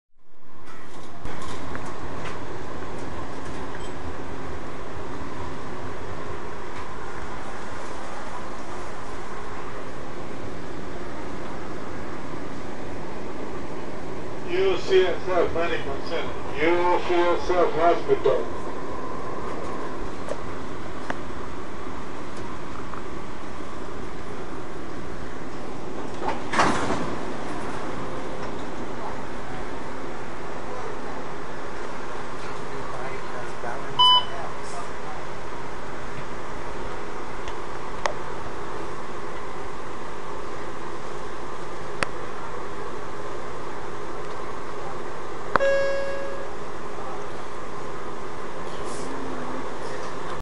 Light Rail Commute Skippy
Skippy more comfortable riding the train now-a-days.